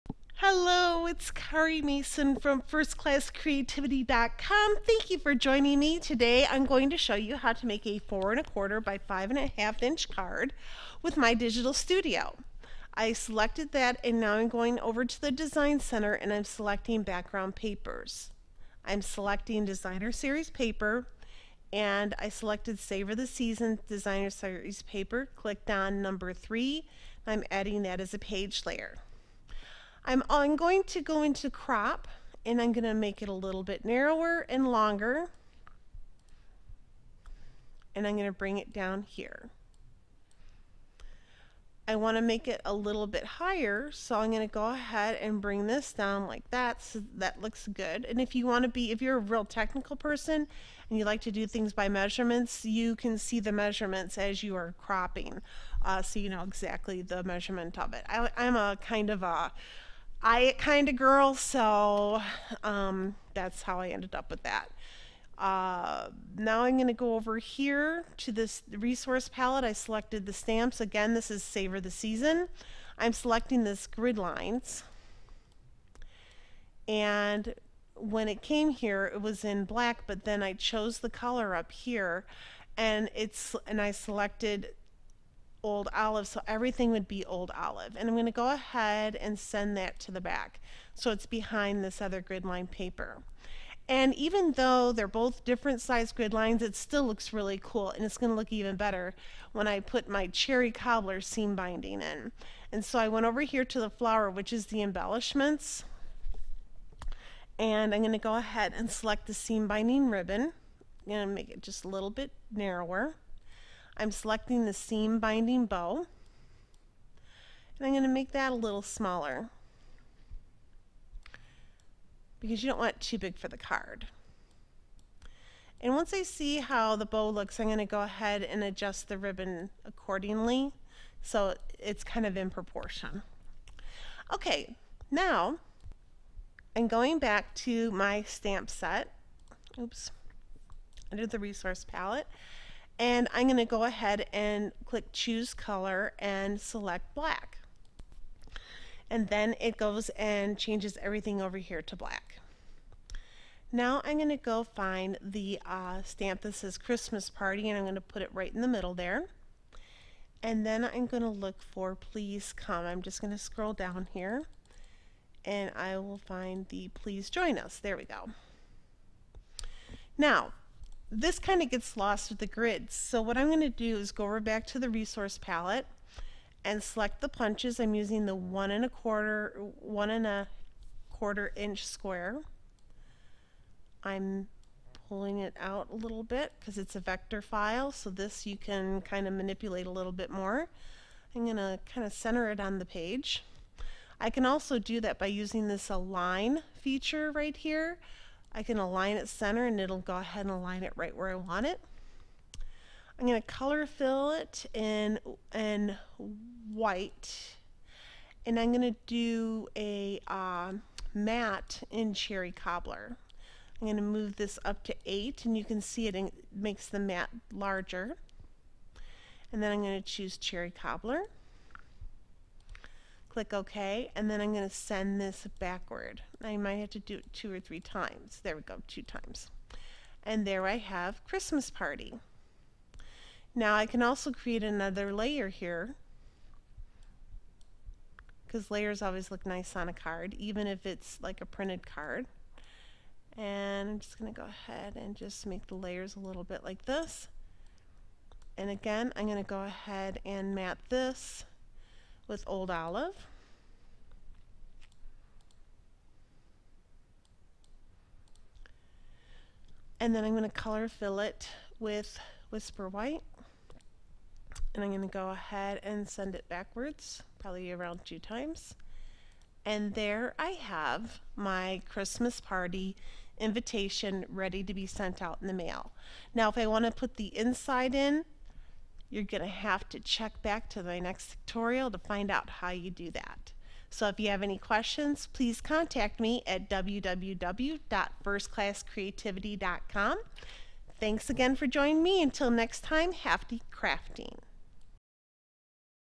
Screencast by 1stClassCreativ from Screenr.com
Savor the Season Stamp Brush Set makes a beautiful Christmas Party Invitation.